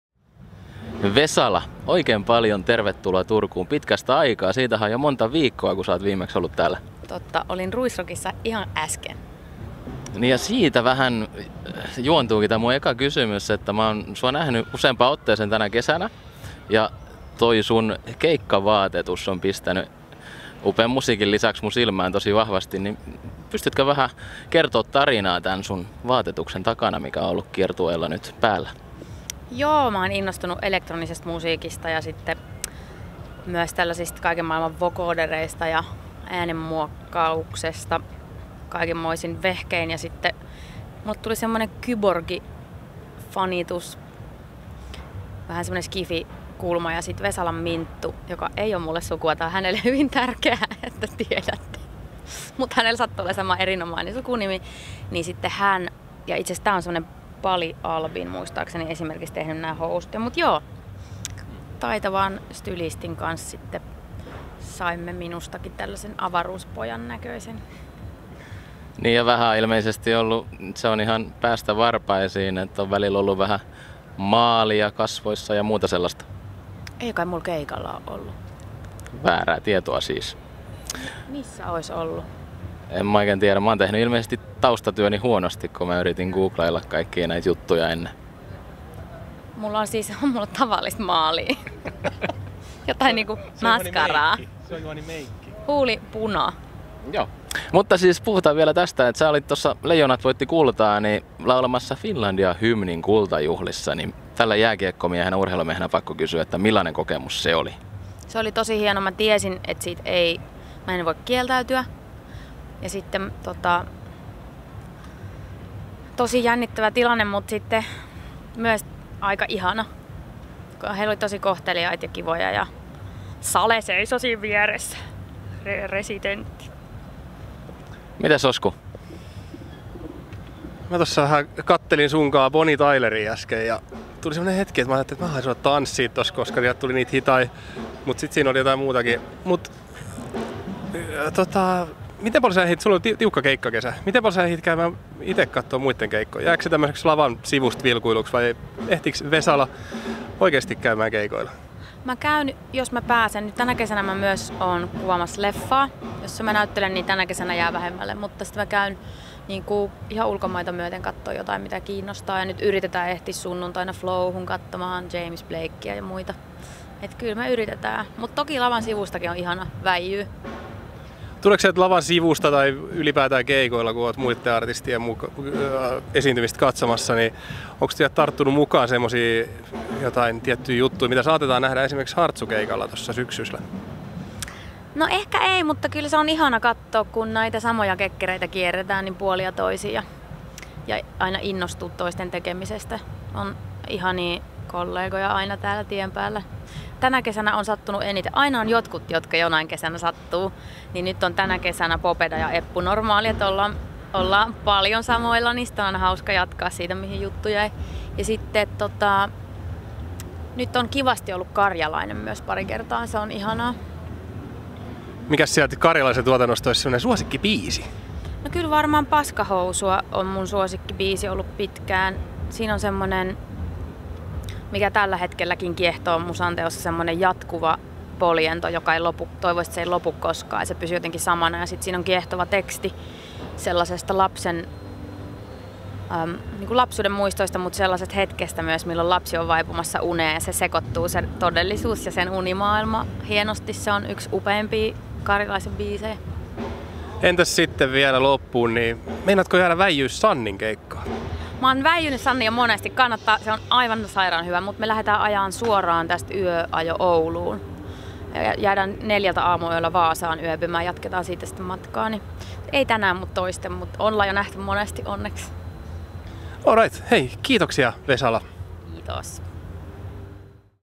DBTL 2019 - Vesala
Juhanin ja Oskun haastattelussa käväissyt Vesala kertoo mm. siitä, ehtiikö esiintyvä artisti nauttia muiden bändien keikoista.

🎥: Tero Jaakkola / DBTL 2019